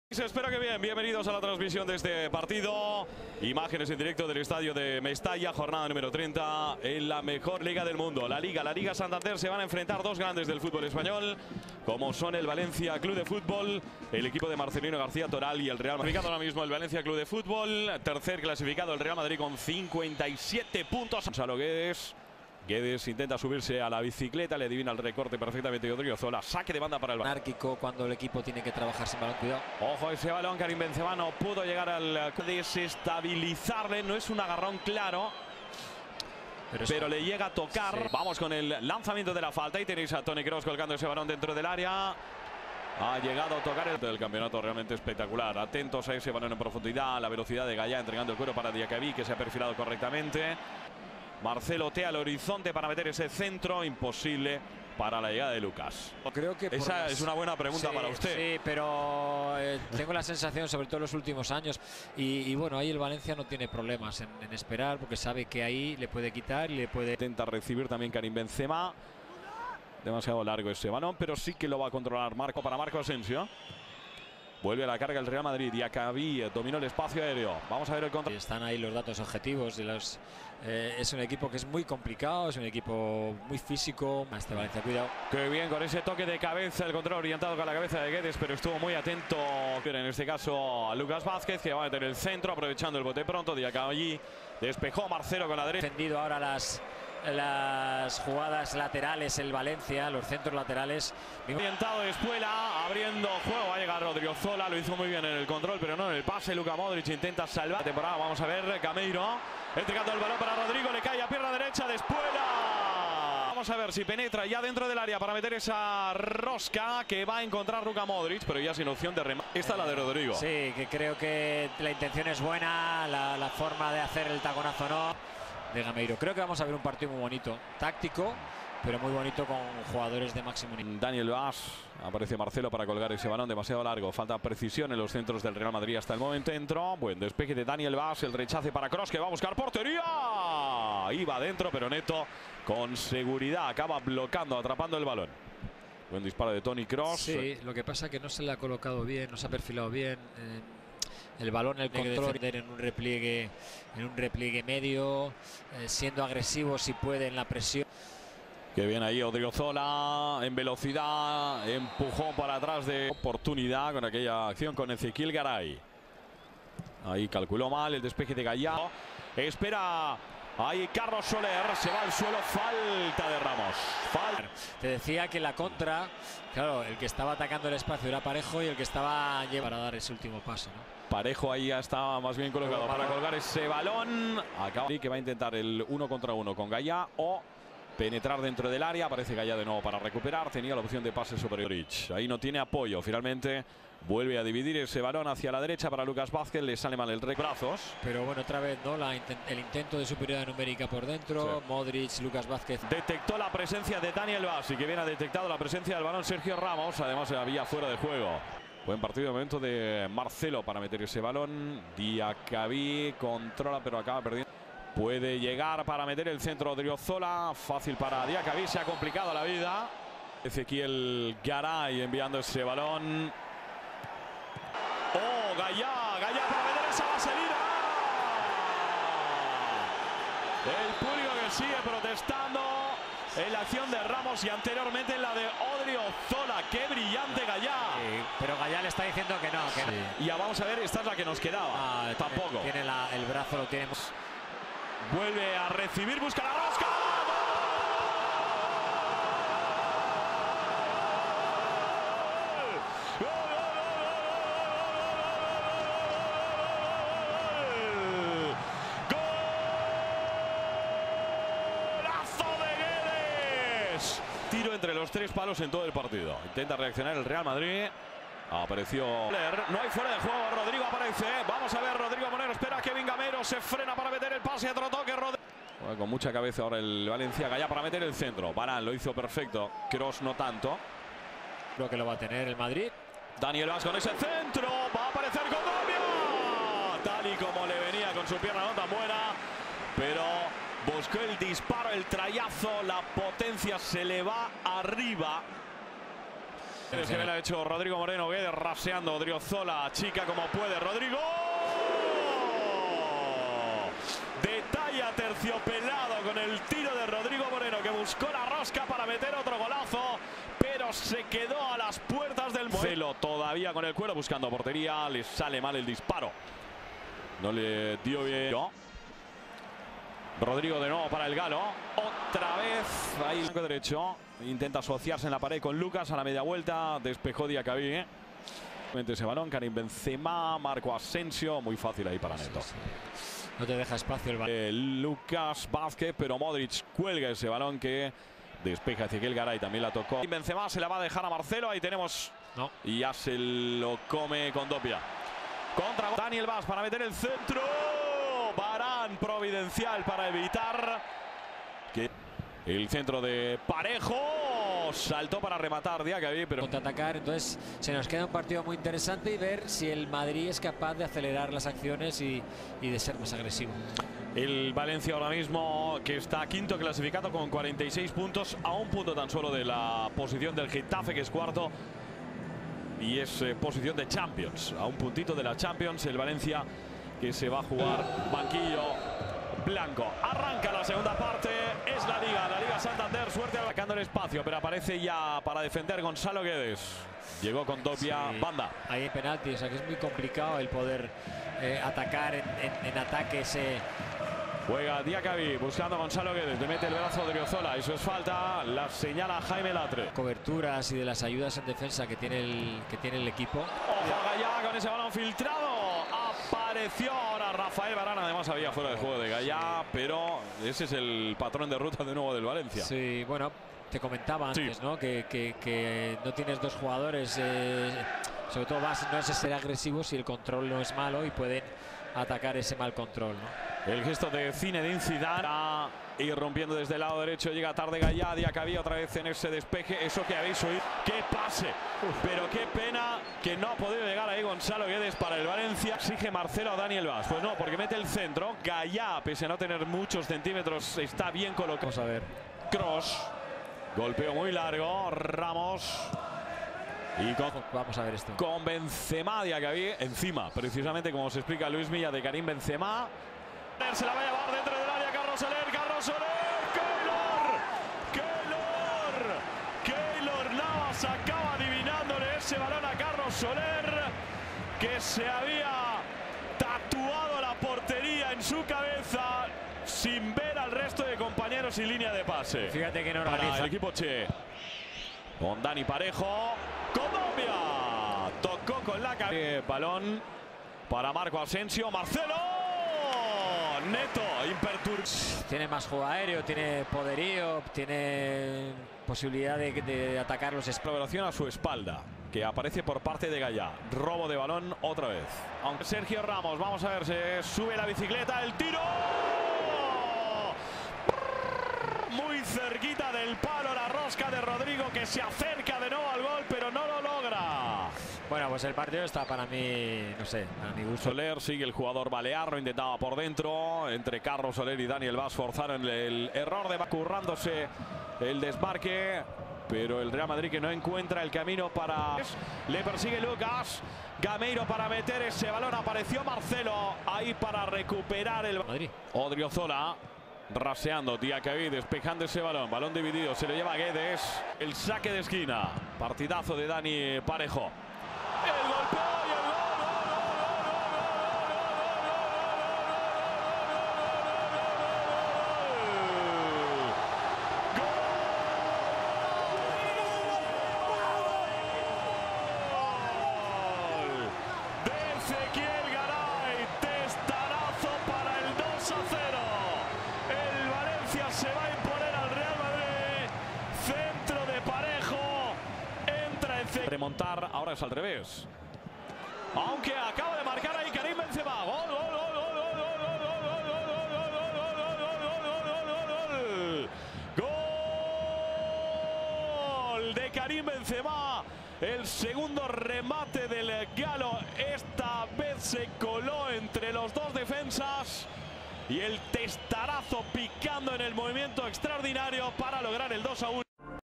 レアル、ジダン体制での初黒星！ リーガ制覇は絶望的！ ラ・リーガ 第30節 バレンシア 2-1 レアルマドリード
ベンゼマが終了間際に1点返すも、レアルはジダン体制での初黒星！　2位とも勝点5差となり、リーガ制覇は絶望的！　ラ・リーガ　第30節　バレンシア 2-1 レアルマドリード